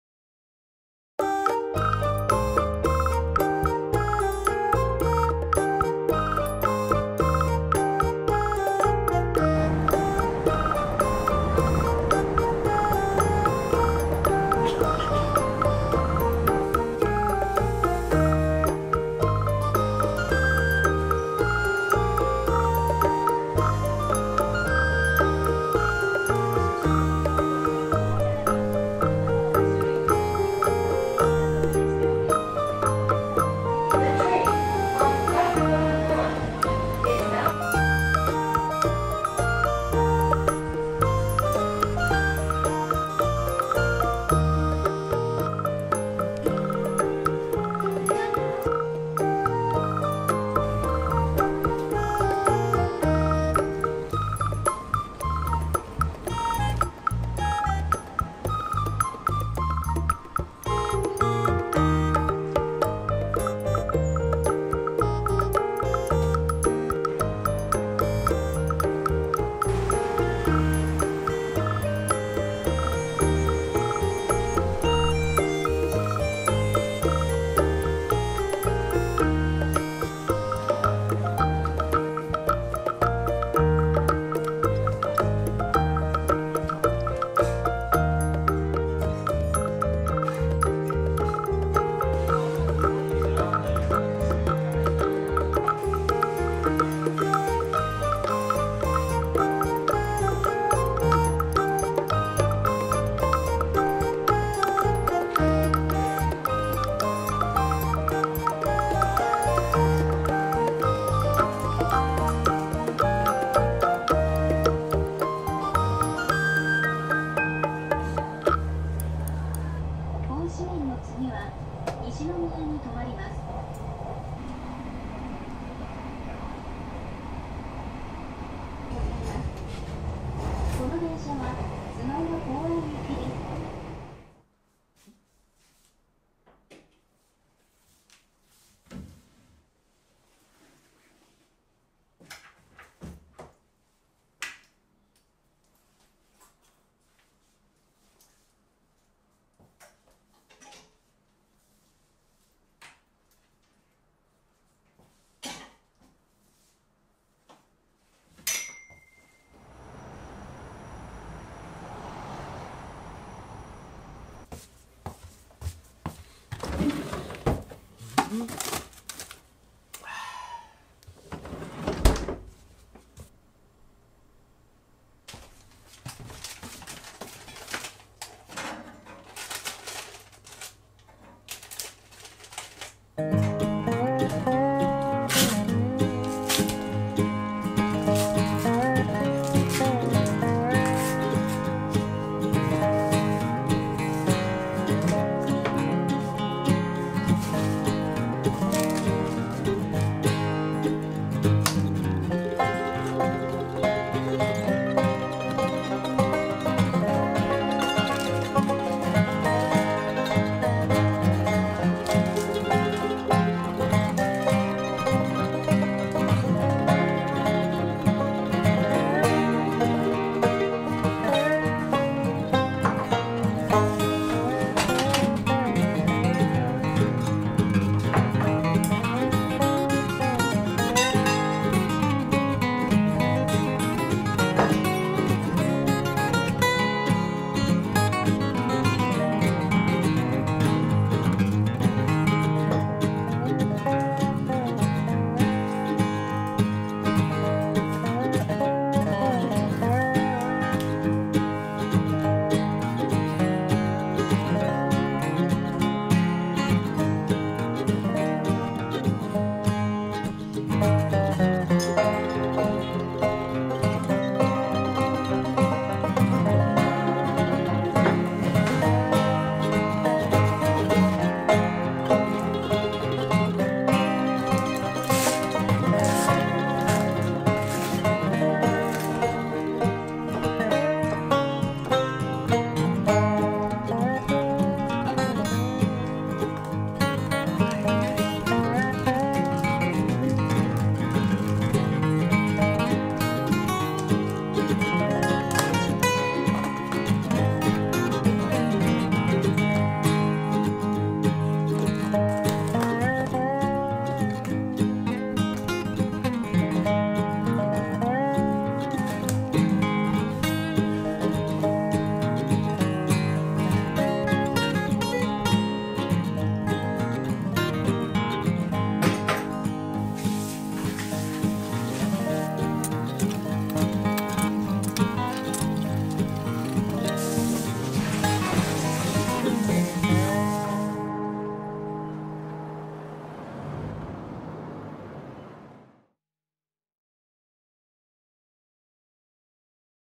友達と大阪の街をプラプラ
友達から大阪の街プラプラしないかと連絡があり
大阪駅で待ち合わせして本当にプラプラ
行くあてもなく　見るものもなく
本当にプラプラ　たまにはいいですね。